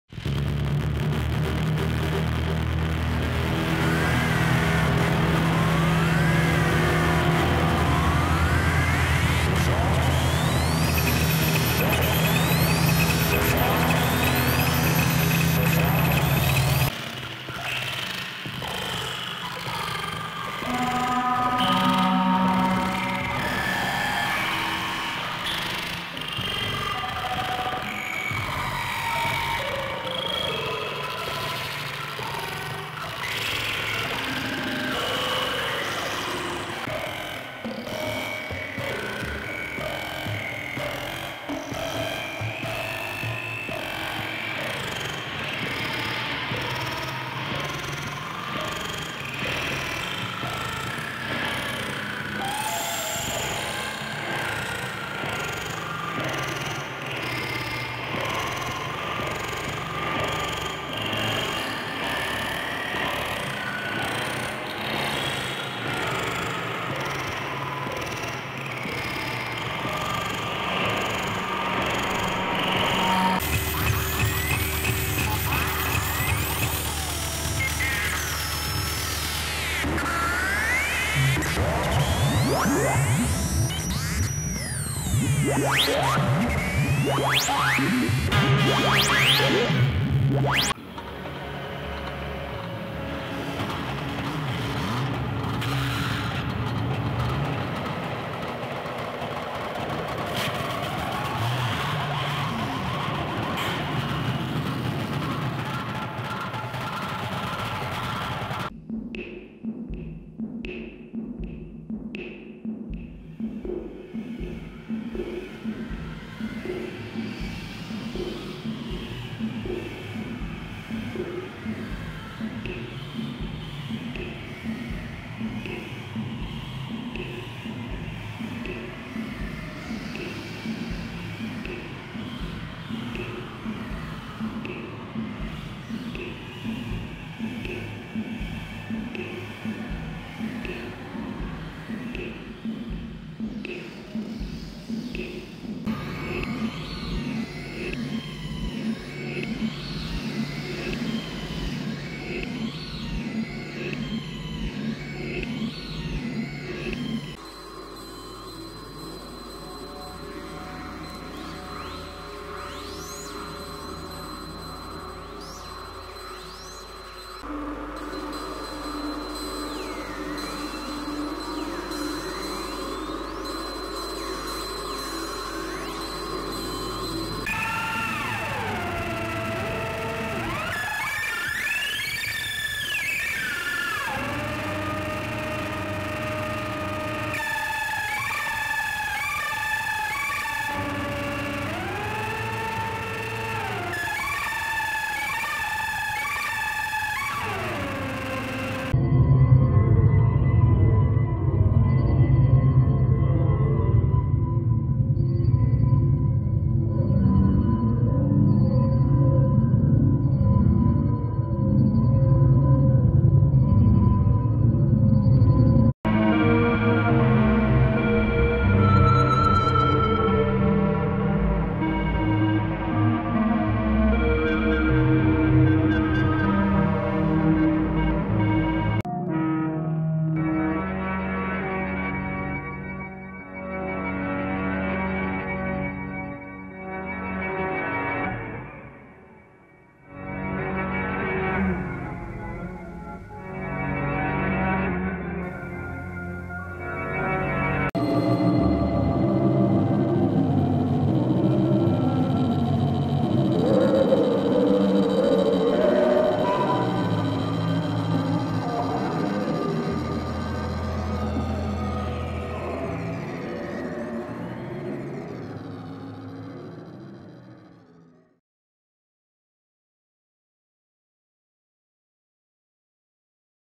Transparent GIF Sticker Showcase Nuggets
Originally posted to Instagram, these clips display transparent GIFs (stickers) made by me that you can use in your social media (not that it does me any good). The sky and the sea are full of bling for your sarcophagus.

Art and music by Colin Raff